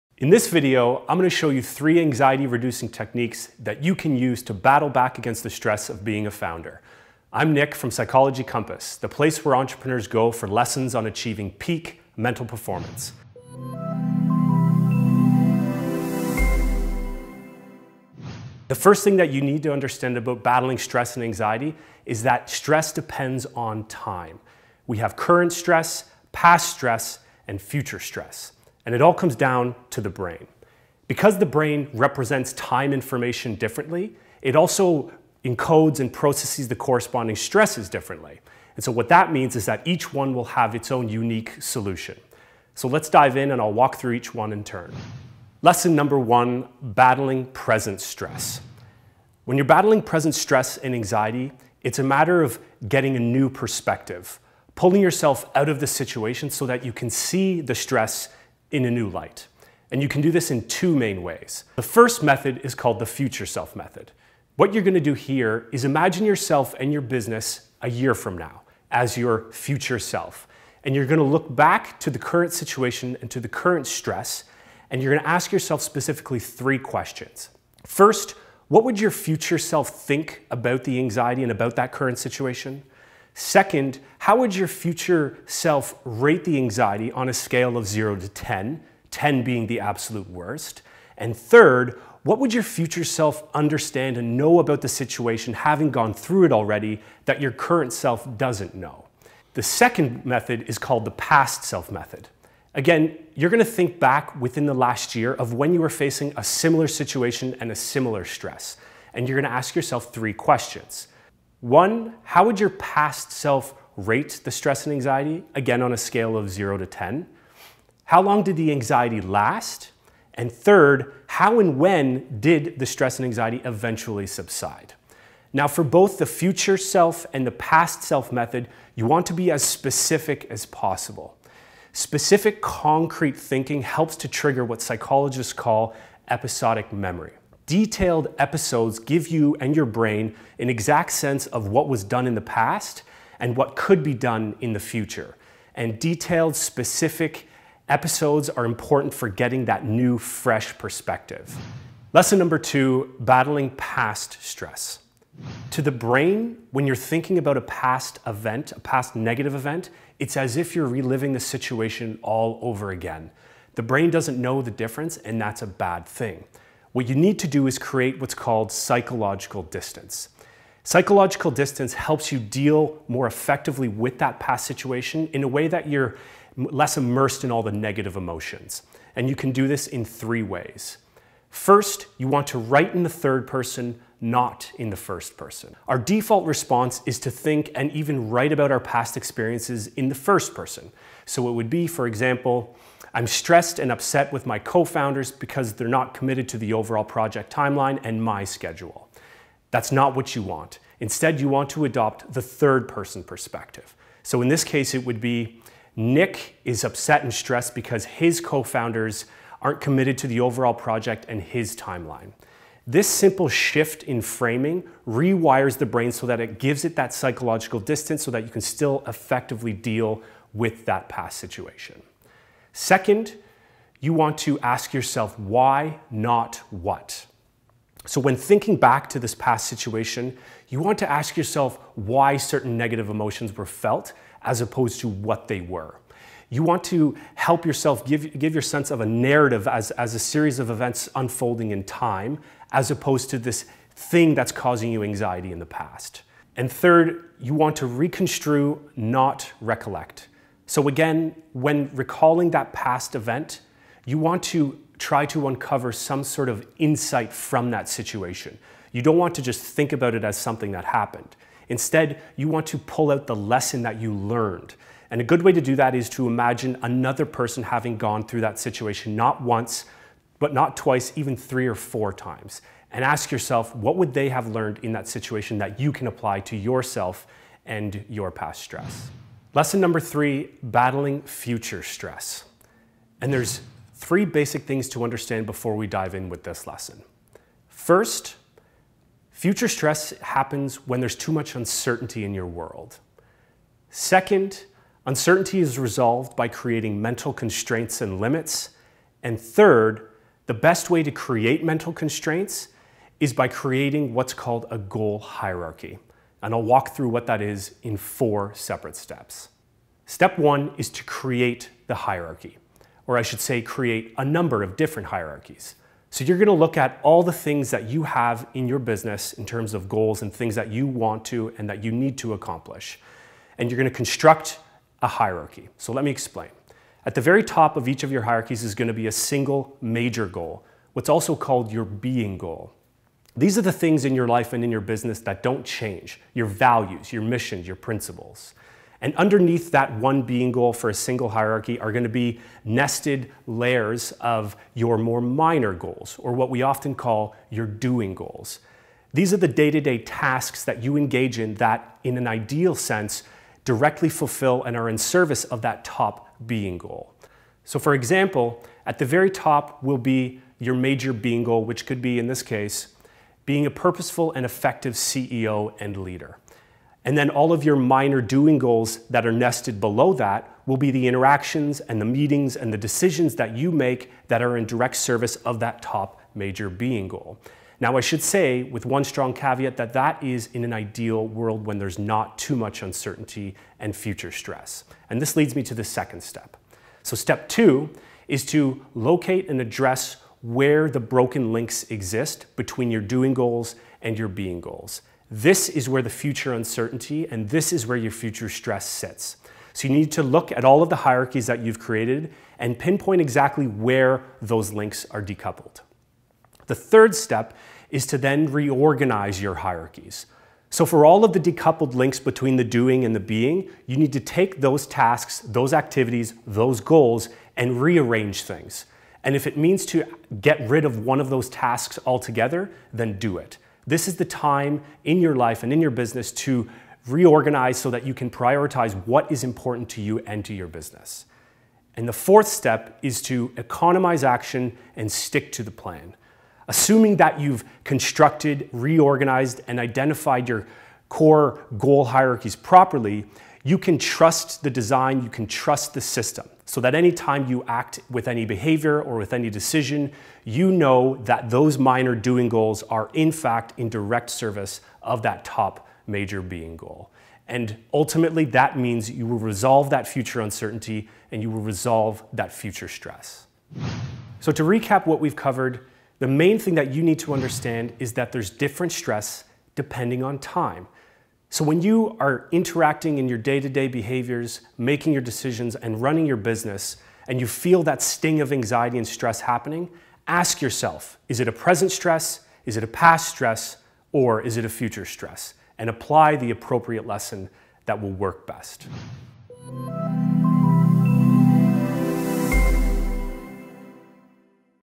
Overcoming Anxiety: Scientific Tactics to Cope with Stress & Anxiety
In this video you'll learn several scientifically proven techniques to overcome anxiety and stress.

What's key in dealing with stress is first understanding if your stress is a Present, Past or Future stress. Each type requires its own method to overcome, according to research in psychology and neuroscience.

With that, here's a preview of what we'll cover in today's video.

First you'll learn two methods to overcome "Present stress" by changing your perspective on time.

Then we'll show you three ways to cope with "Past stress", using what's called psychological distance.

Then we'll share a thinking framework that helps you overcome "Future stress".

In short we'll share techniques with you that you can always use, no matter what type of stress and anxiety you're faced with.

Here are all the citations for this video:

Kross, E., & Ayduk, O. (2011). Making meaning out of negative experiences by self-distancing. Current Directions in Psychological Science, 20, 187-191.

Hirsh, J.B., Mar, R.A., & Peterson, J.B. (2012). Psychological entropy: A framework for understanding uncertainty-related information. Psychological Review, 119, 304-320.

Quoidbach, J., Wood, A. M., & Hansenne, M. (2009). Back to the future: The effect of daily practice of mental time travel into the future on happiness and anxiety. The Journal of Positive Psychology, 4, 349-355.

Guerin, B. (2001). Replacing catharsis and uncertainty reduction theories with descriptions of historical and social context. Review of General Psychology, 5(1), 44-61.

Elliot, A. J., & McGregor, H. A. (1999). Test anxiety and the hierarchical model of approach and avoidance achievement motivation. Journal of Personality and Social Psychology, 76, 628-644.

Hobson, N. M., Bonk, D., & Inzlicht, M (2017). Rituals decrease the neural response to performance failure. PeerJ, e3363, doi: 10.7717/peerj.3363.